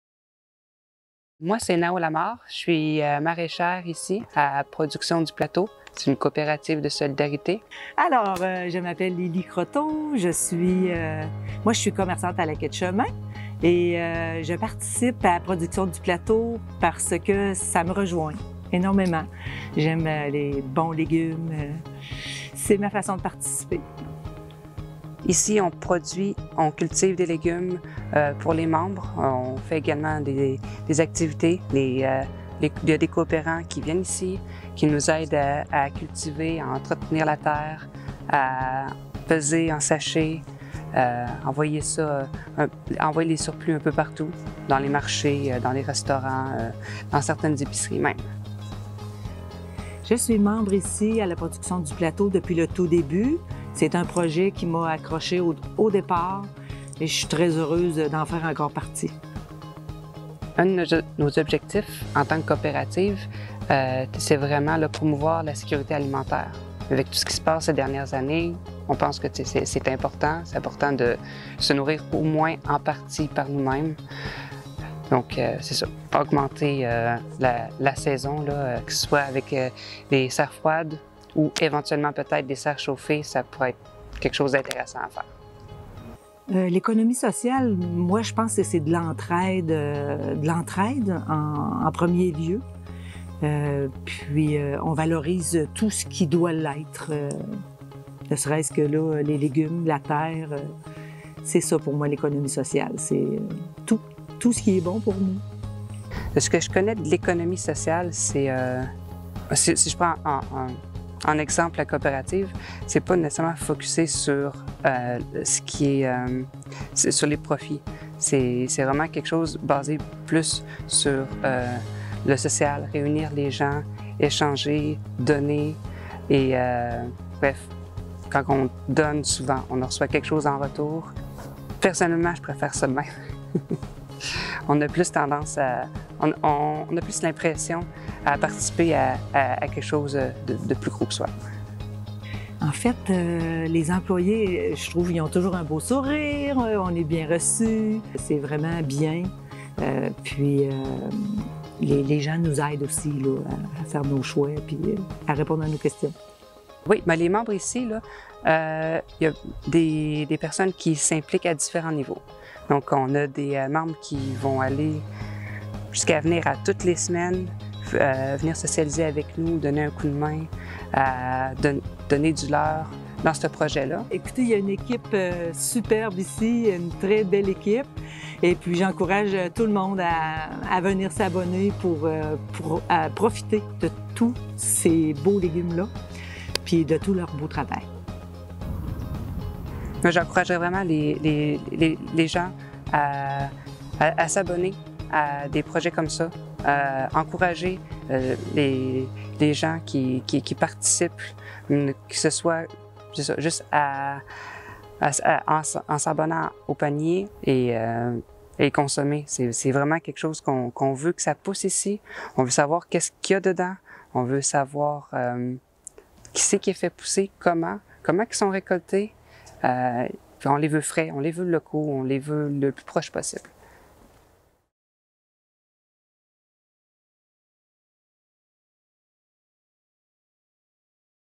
La TRÉSCA présente : Production Du Plateau
Production du Plateau
Version sous-titrée français

La coopérative de solidarité Production du plateau est une entreprise d’économie sociale qui est née d’une initiative citoyenne en 2018.
Inspirée d’une expérimentation locale de production maraîchère ayant pour mission la réinsertion sociale et professionnelle des travailleurs (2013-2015), un groupe de citoyen.ne.s a poursuivi une démarche dans le but de doter la région d’un équipement collectif afin de poursuivre la production maraîchère.

Les missions :
1. Donner accès à des produits maraîchers frais, écologiques,
produits localement à un coût équitable, selon un modèle de
permaculture;

2. Augmenter la sécurité alimentaire des gens de la région en
agissant sur les déterminants collectifs.
Les valeurs communes sont :
- développement durable
- respect
- ouverture
- entraide – mutualisation des ressources
- autonomie

Réalisation et montage : Lucie Perdreau - TRÉSCA

Avril 2022

Production du Plateau

TRÉSCA